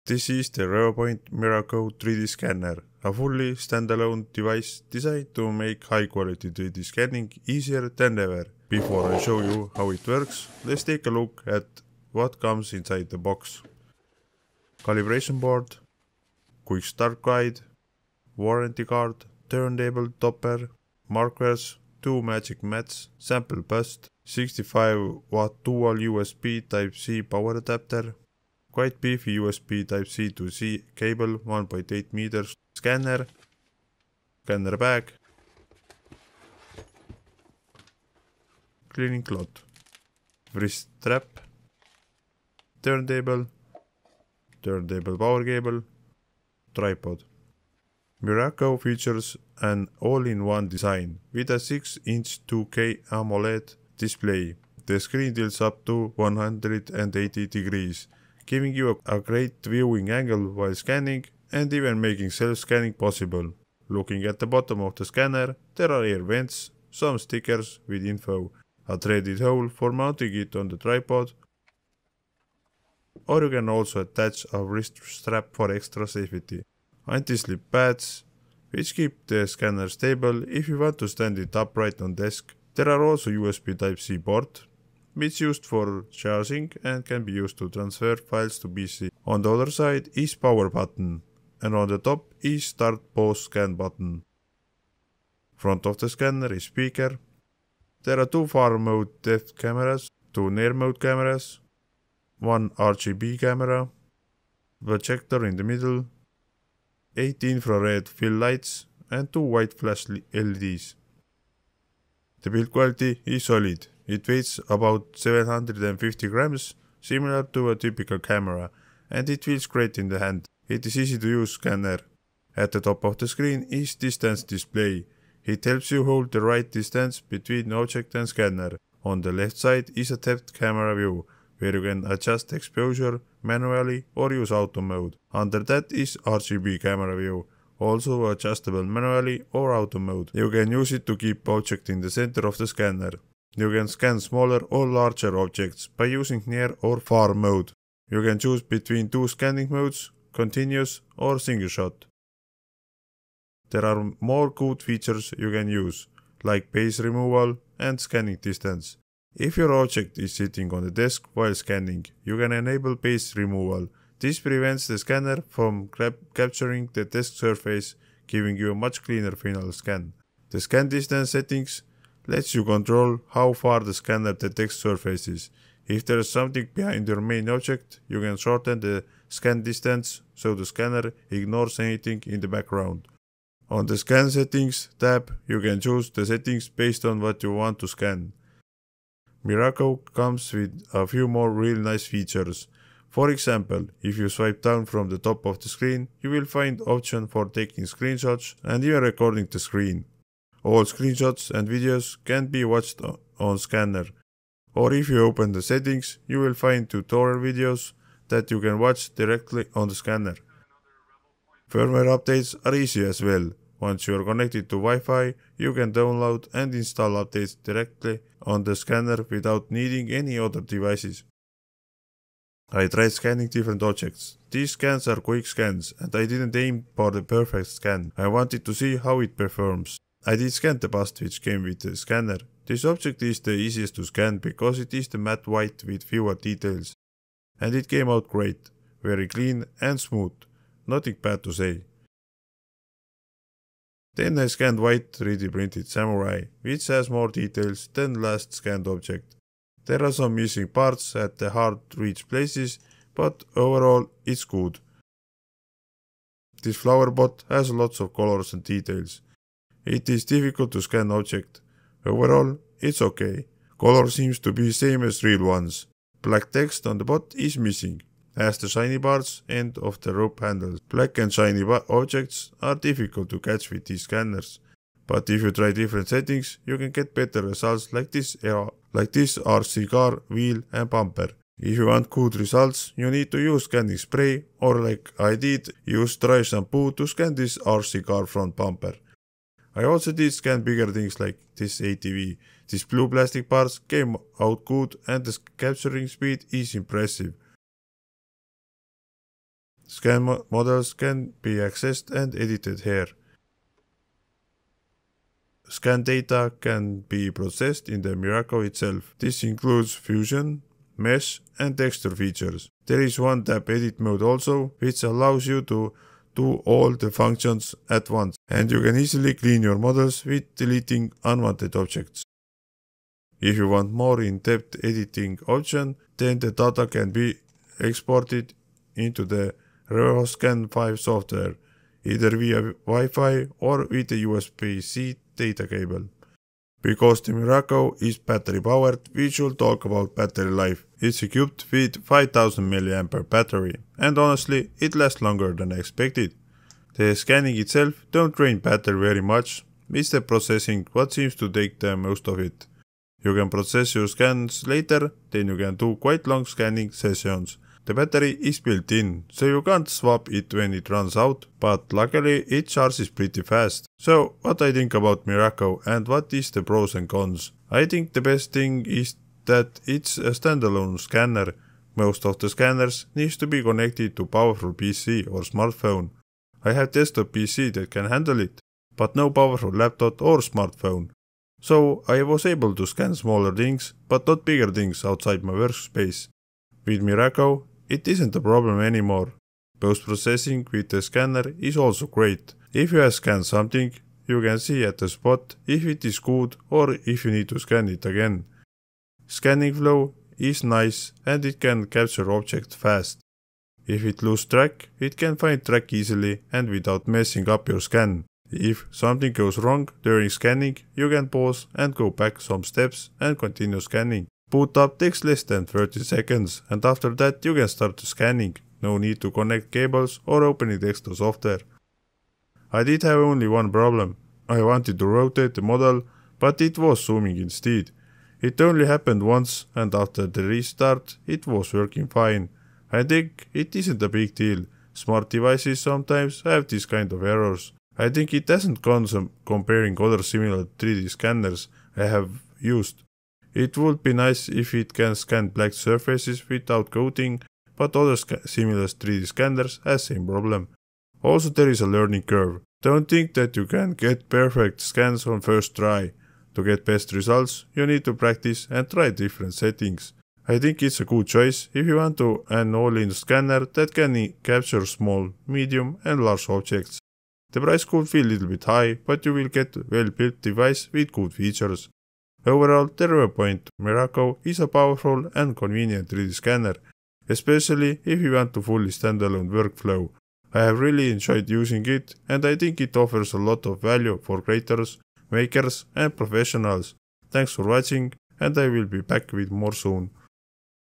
Revopoint Miraco 3D Scanner - The Best Handheld Scanner I've Tested
In this video, I review the Revopoint MIRACO 3D Scanner — a fully standalone scanner that lets you capture, process, and export 3D models directly on the device.

Use code【REVOPR3DPB】to get your Revopoint Scanner!